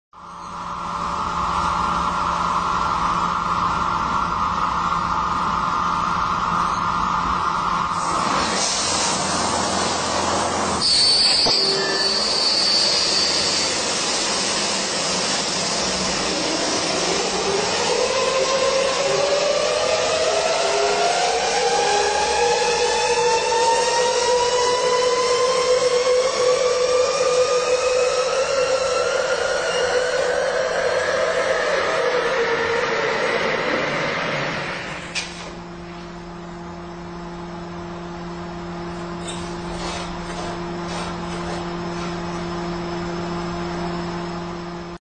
of air brake test
Today's largest passenger aircraft Airbus A380 aircraft which holds the distinction of being the slope of the braking test, which consists of the aircraft brake discs burned due to high energy